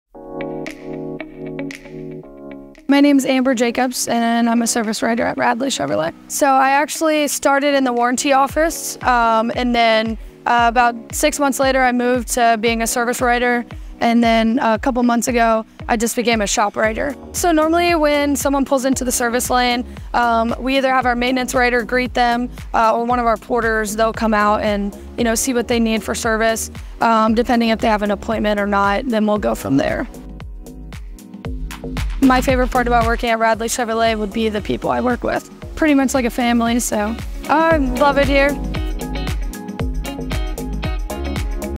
Meet Amber Jacobs - Service Writer at Radley Chevrolet
Meet Amber Jacobs, one of our amazing Service Writers here at Radley Chevrolet! With her expertise and unwavering commitment to customer satisfaction, Amber guarantees a memorable experience for every customer walking through our doors. Swing by our shop or visit us online today to experience the Radley Chevrolet difference for yourself!